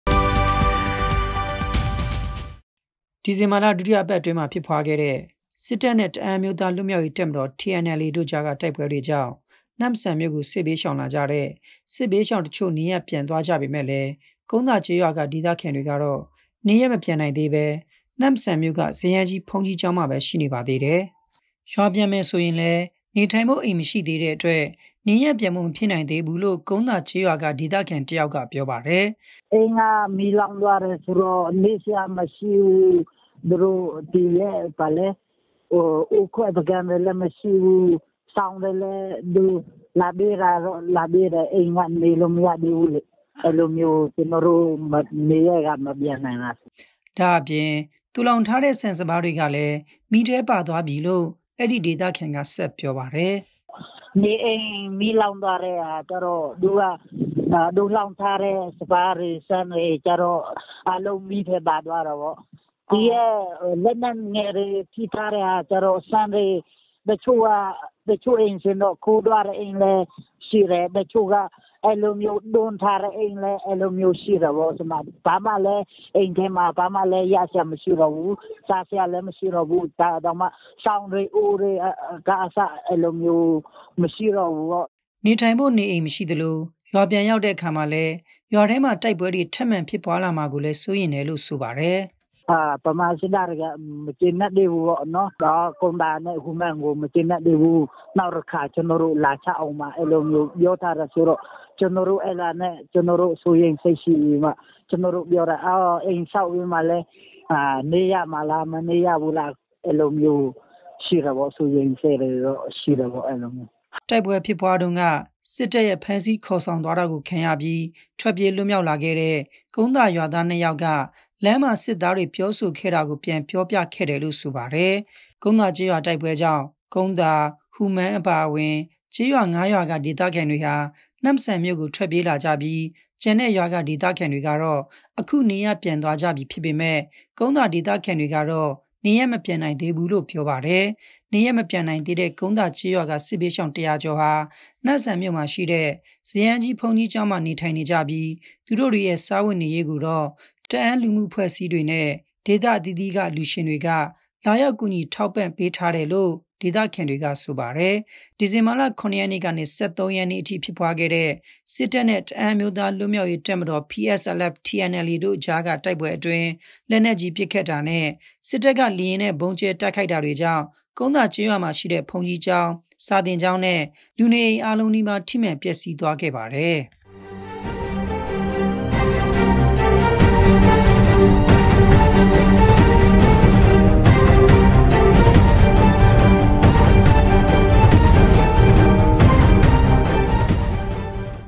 ကုန်းသာ စစ်ရှောင်တွေ အိမ်မပြန်နိုင်သေးတာ ဘာကြောင့်လဲ
ဒီဇင်ဘာလ ဒုတိယပတ်မှာ ဖြစ်ပွားခဲ့တဲ့ စစ်တပ်နှင့် တအန်းအမျိုးသား လွတ်မြောက်ရေး တပ်မတော် TNLA တို့အကြားက တိုက်ပွဲတွေကြောင့် နမ့်ဆန်မြို့ကို စစ်ဘေးရှောင်လာကြတဲ့ စစ်ဘေးရှောင်တချို့ နေရပ်ပြန်သွား ကြပေမယ့်လည်း ကုန်းသာကျေးရွာက ဒေသခံတွေကတော့ နေရပ်မပြန်နိုင်သေးဘူးလို့ စစ်ရှောင်တွေက ပြောသည်။